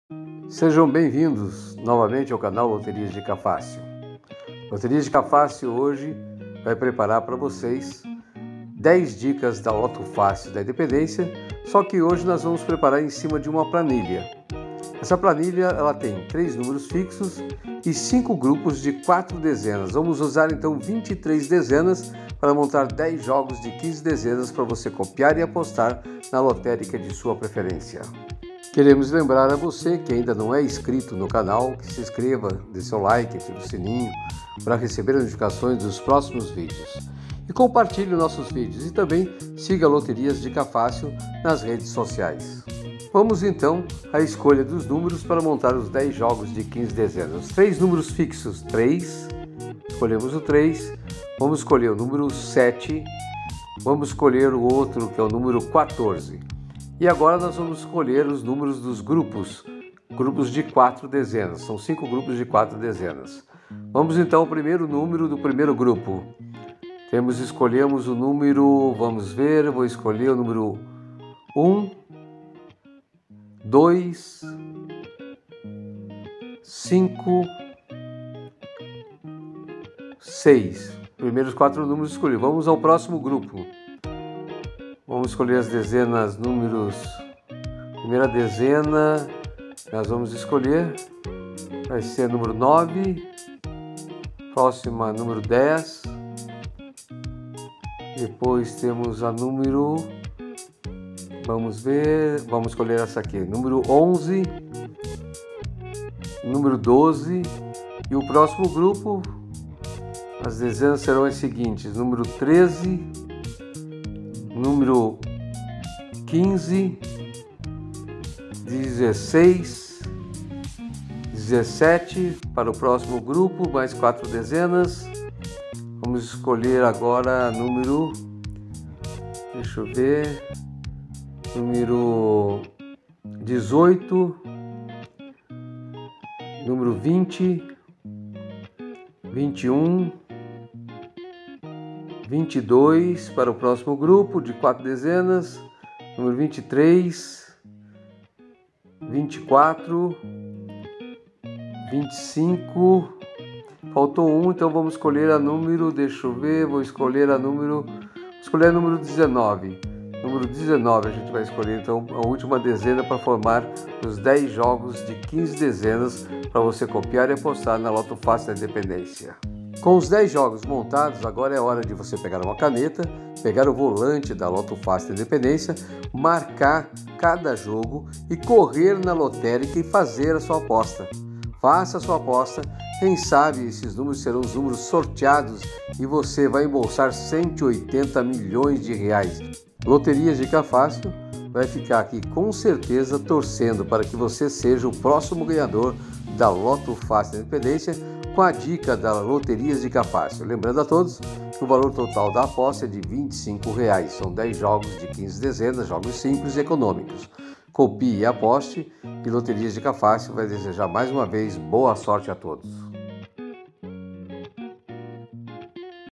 [Dica]🍀Planilha Lotofácil Independência 2610 desdobramento [TRÊS DEZENAS FIXAS] 180 Milhões 🌽🌽🌽🌽🌽🌽
Usamos uma Planilha
Desdobramento 23 Dezenas
3 Dezenas fixas
10 jogos econômicos de 15 dezenas
Valor total da aposta R$25,00 reais
Copiar e apostar na Lotérica de sua preferência
Boa Sorte!🍀🍀🍀